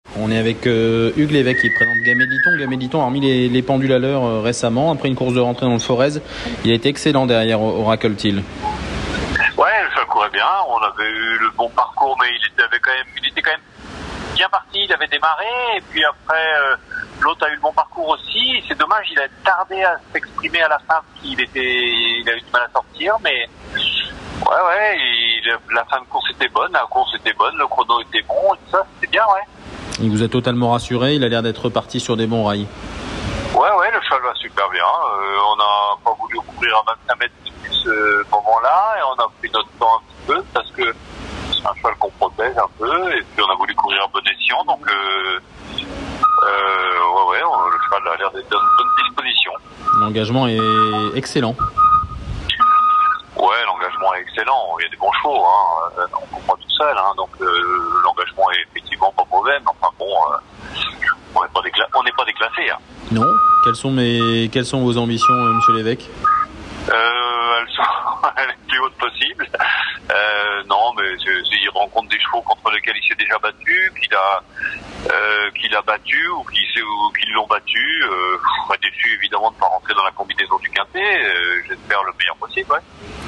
Hughes Levesque, entraîneur de Gamay de l'Iton (02/03 à Paris-Vincennes)
Hughes Levesque, entraîneur de Gamay de l'Iton (11) nous parle de ses chances dans le Quinté+, le Prix Emile Allix Courboy sur l’hippodrome de Paris-Vincennes le 02/03/2023.